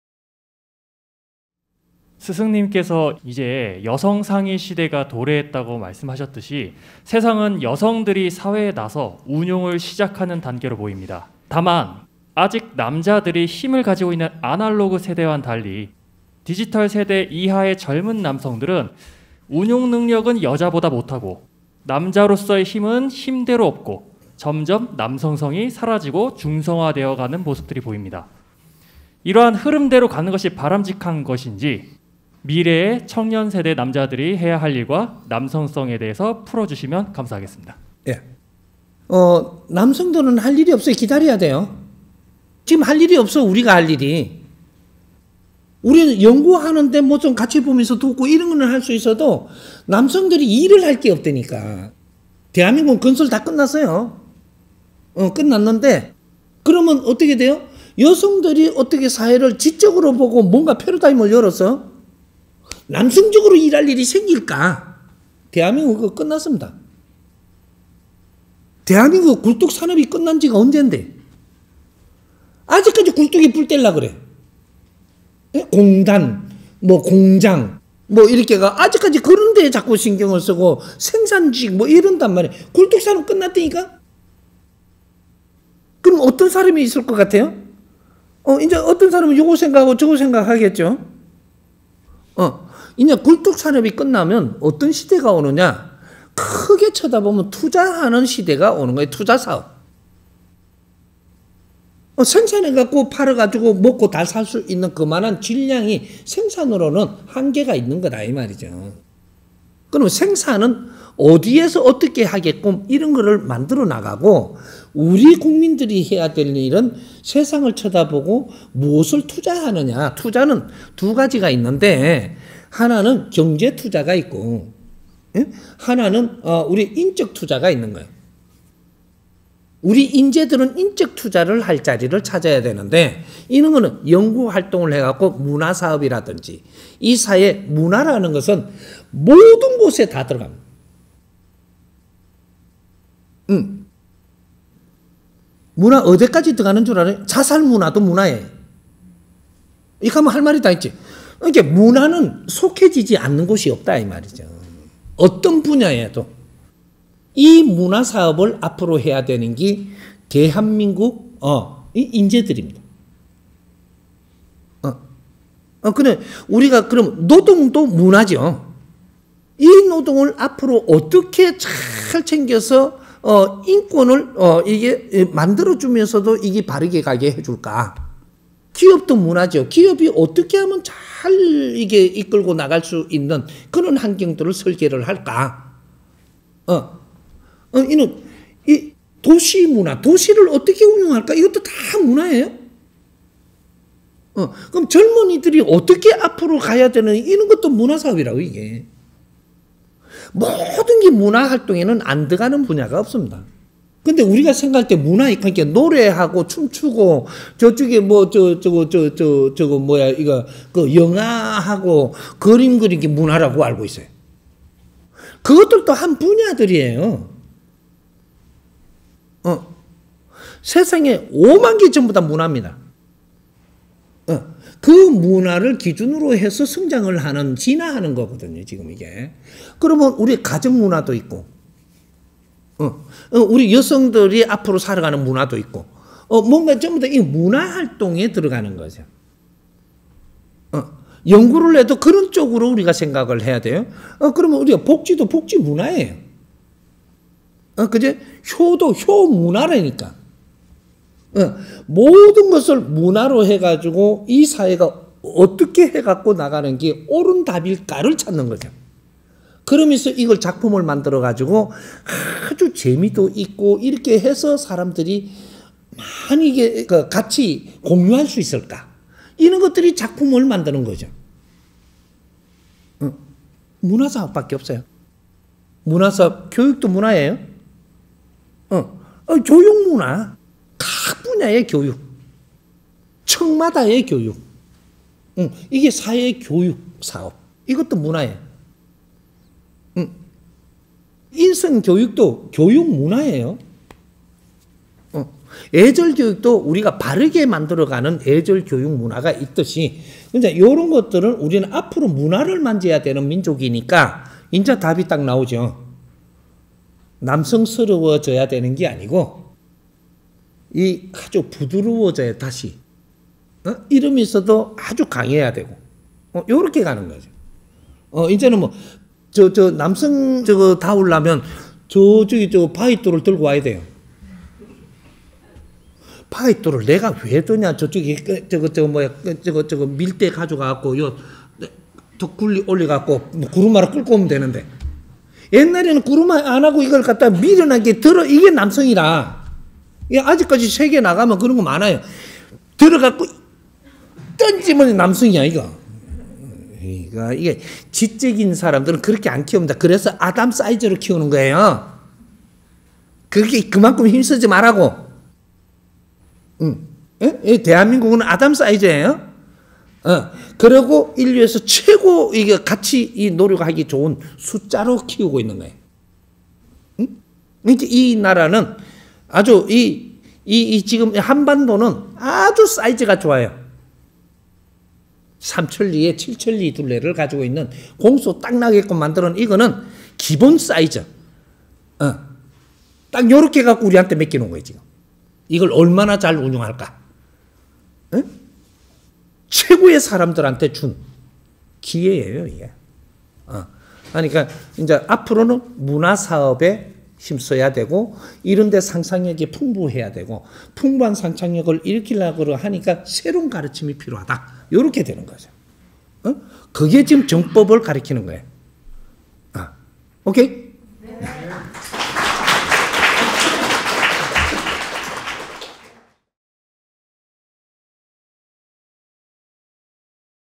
[천공 정법]12719강 중성화되어가는 젊은 남자들의 역할과 미래[홍익인간 인성교육]
질문: 스승님께서 이제 여성 상위시대가 도래했다고 말씀하셨듯이 세상은 여성들이 사회에 나서 운용을 시작하는 단계로 보입니다. 다만, 아직 남자들이 힘을 가지고 있는 아날로그 세대와는 달리 디지털 세대 이하의 젊은 남성들은 운용능력은 여자보다 못하고 남자로서의 힘은 힘대로 없어 점점 남성성이 사라지고 중성화되어가는 모습들이 보입니다. 이러한 흐름대로 가는 것이 바람직한지 미래의 청년세대 남자들이 해야 할 일과 남성성에 대해서 풀어주시면 감사하겠습니다.

강의 일자: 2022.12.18 수원 / 정법시대: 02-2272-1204